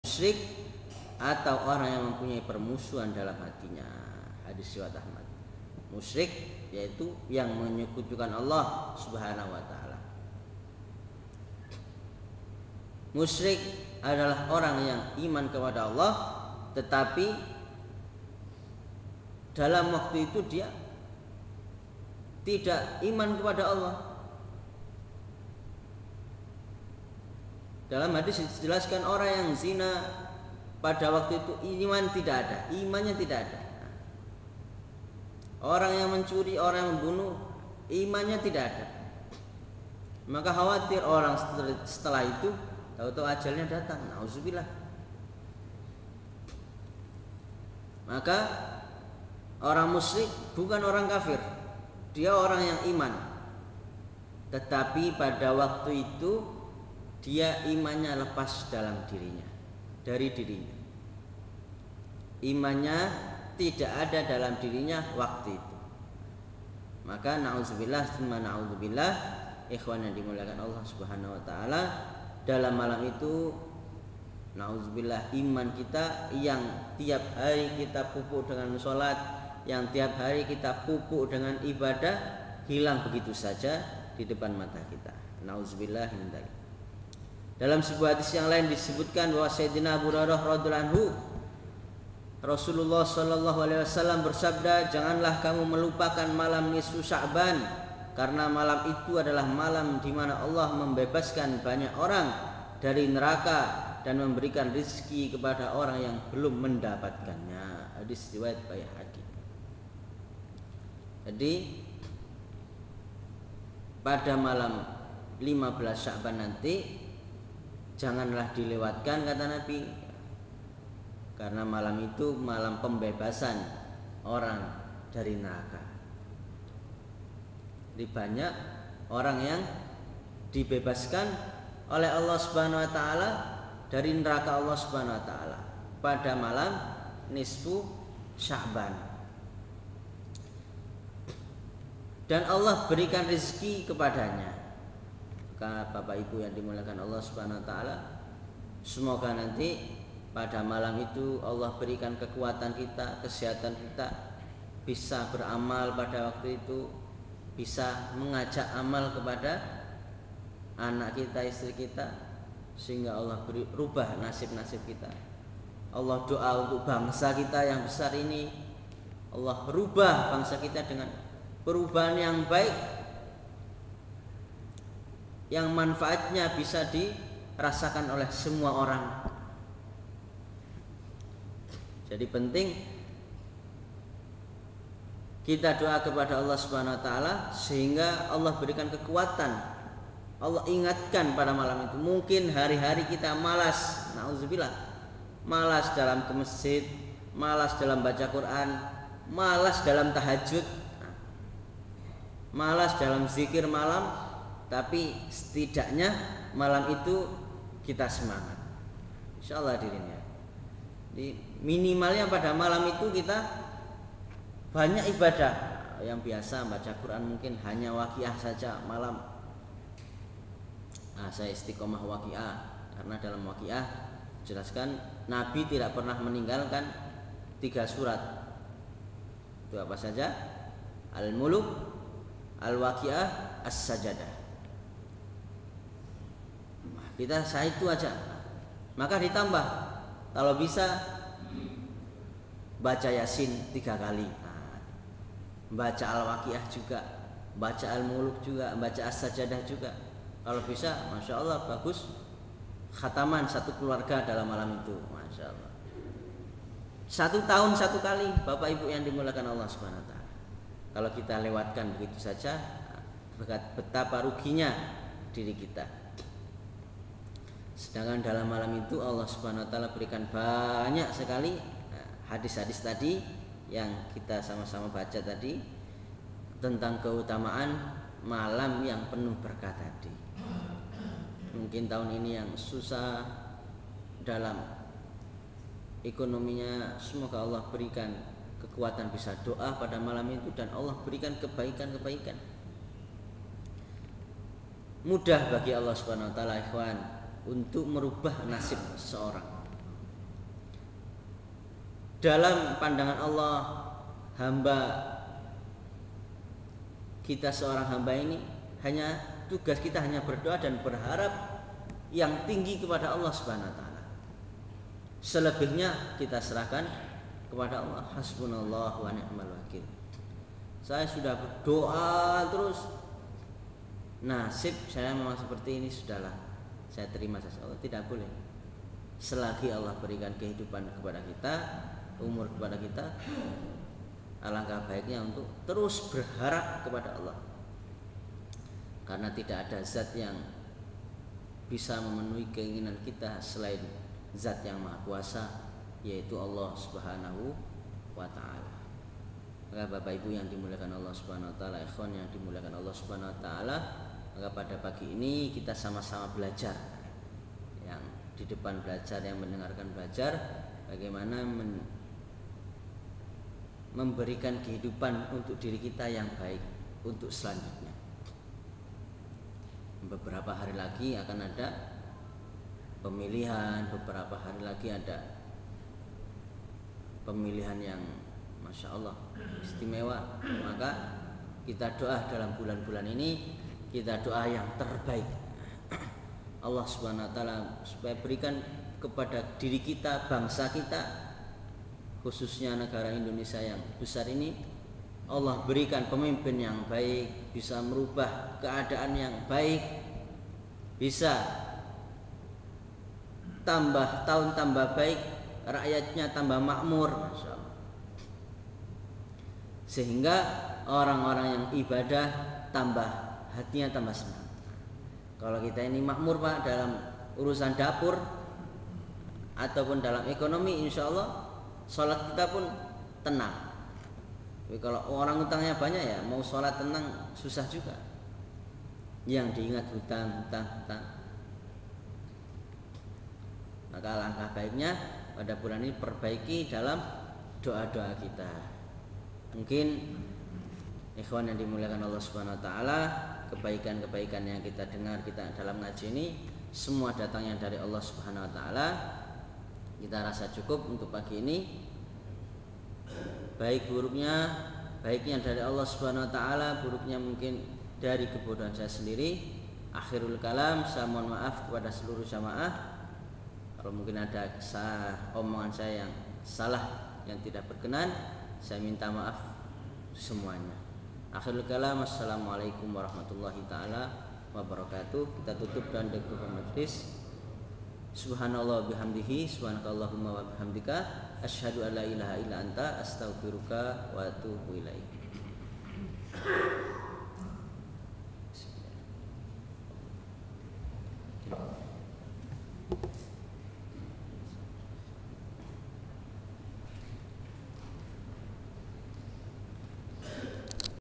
Part 2 | Keistimewaan Bulan Sya'ban | Ust. Ahmad Shiddiq | 10/2/24
Masjid Baitul Haq Ketintang Permai Surabaya
Jadwal Kajian Rutin : Rabu (Ba'da Maghrib) - Sabtu dan Ahad (Ba'da Shubuh)